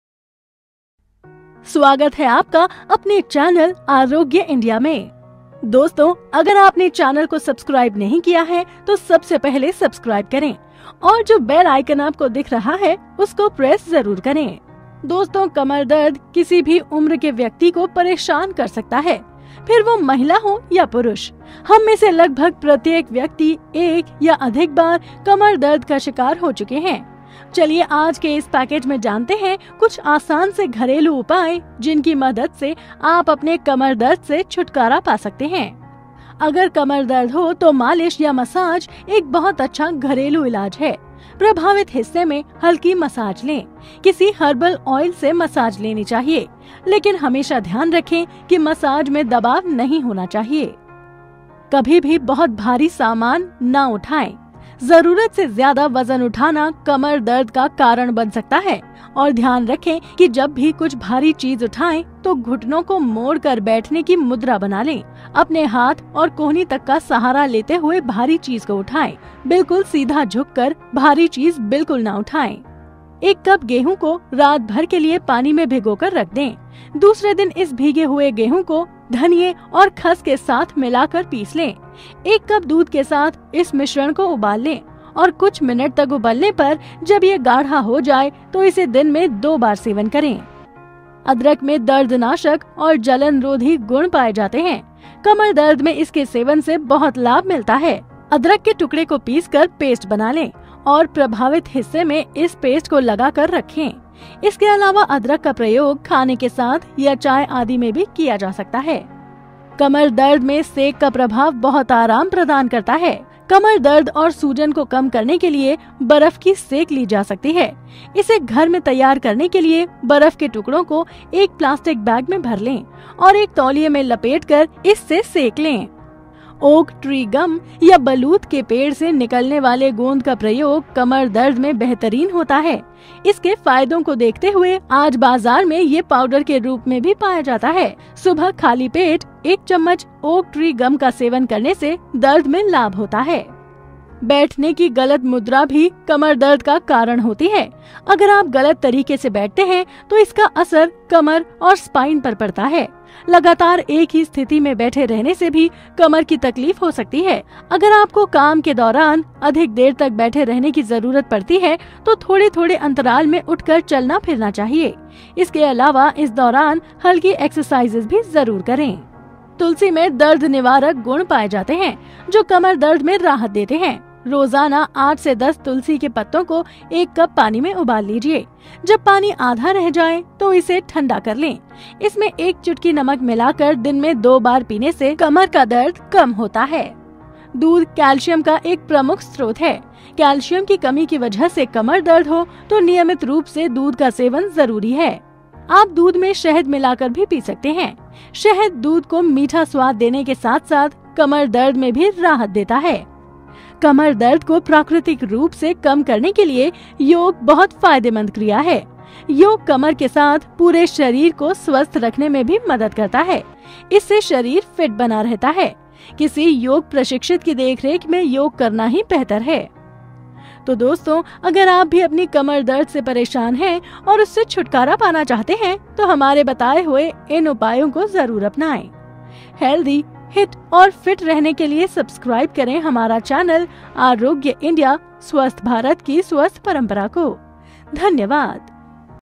3 मिनिट भयंकर से भयंकर कमरदर्द और स्लिप डिस्क का रामबाण इलाज || Arogya India
Copyright : Arogya India